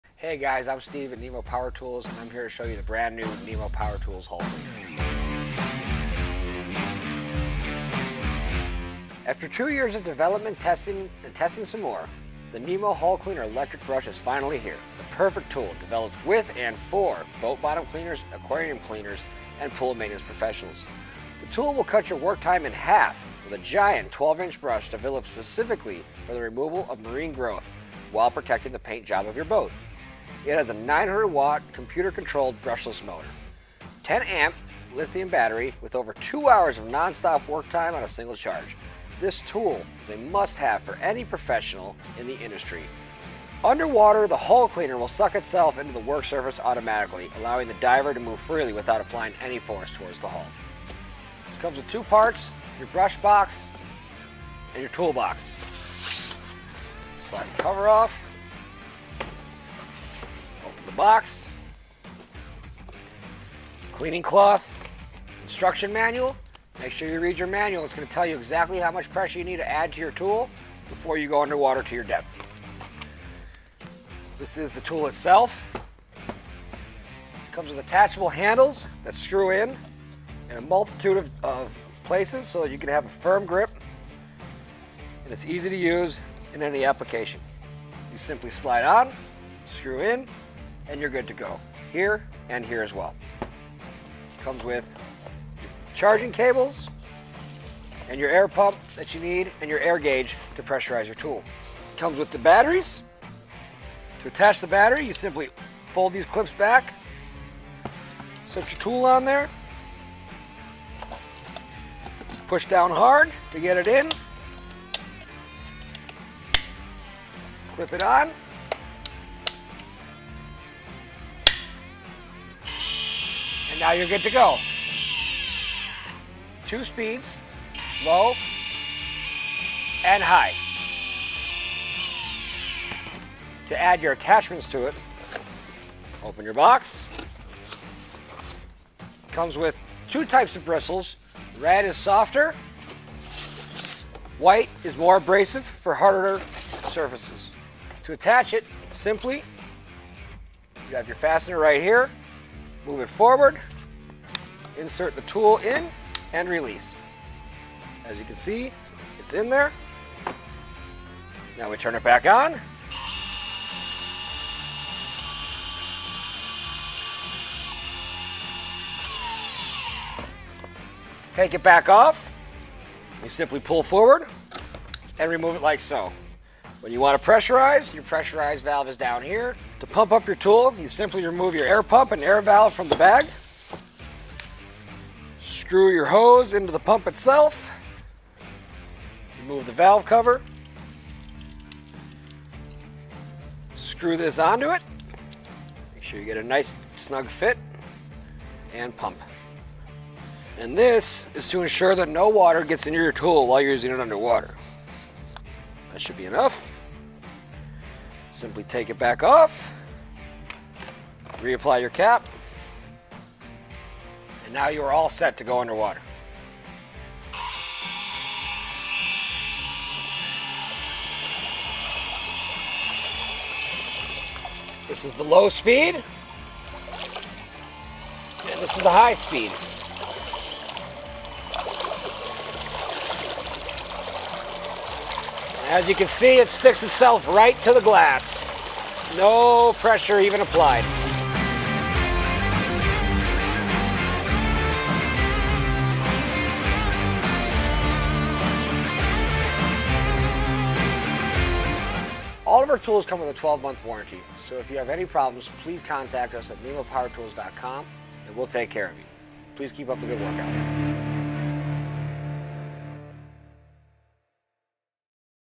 Nemo Power Tools - Hull Cleaner Electric Brush - Intro & Demo
After 2 years of development, testing and testing some more, the Nemo Hull Cleaner Electric Brush is finally here. The perfect tool, developed WITH and FOR boat bottom cleaners, aquarium cleaners and pool maintenance professionals.

Cut your work time in half with a giant 12 inch brush, developed specifically for the removal of marine growth, while protecting the paint job of your boat.

900 watts computer controlled brushless motor, 10 Ah lithium battery with over 2 hrs non-stop work time on a single charge!

This tool is a must have for any professional in the industry.
The tool will suck itself into the work surface automatically, allowing the diver to move freely without applying force towards the hull.

This product includes two 12 inch specialized brushes, and comes in a high quality water proof "hard case".